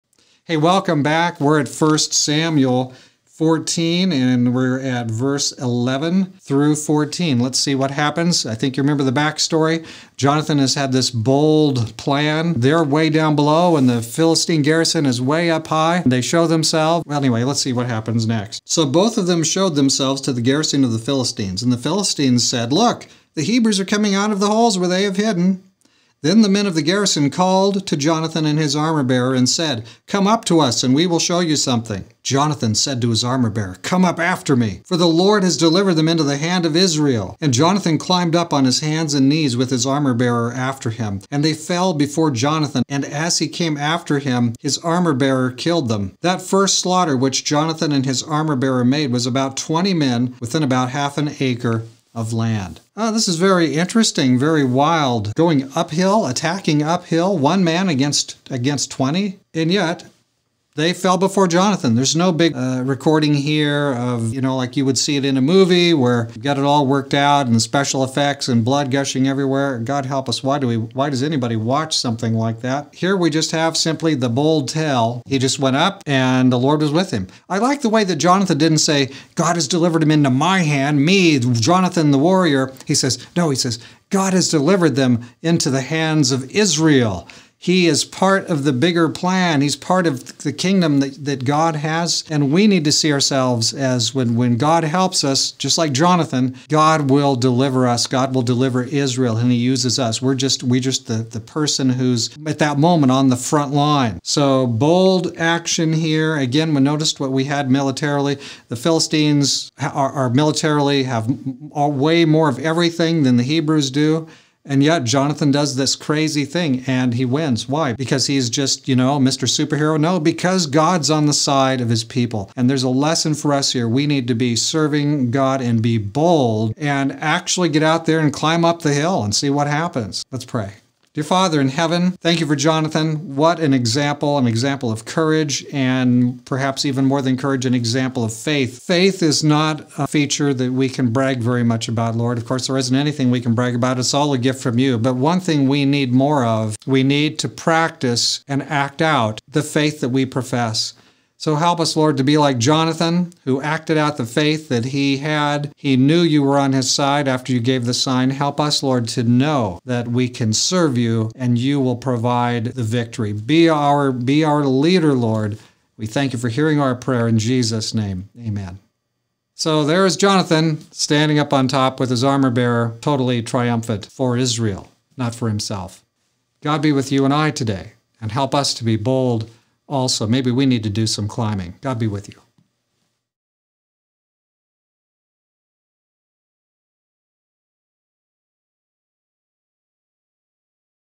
1 Samuel 14:11-14 Jonathan goes Up and Over!
Jonathan is intrepid. He will attack at 20 to 1 odds, and up a cliff face. What happens?